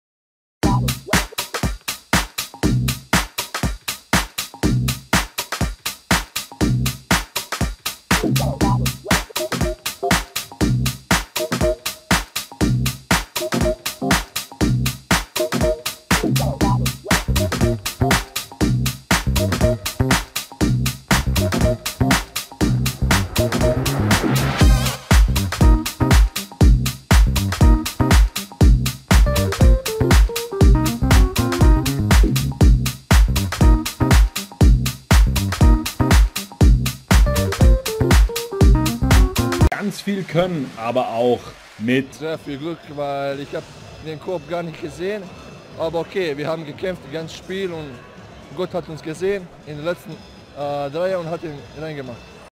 Nihad Djedovic: Amazing performance vs Brose Baskets
Nihad Djedovic: Amazing performance vs Brose Baskets  (FC Bayern Muncih Basketball 84 - 74 Brose Baskets)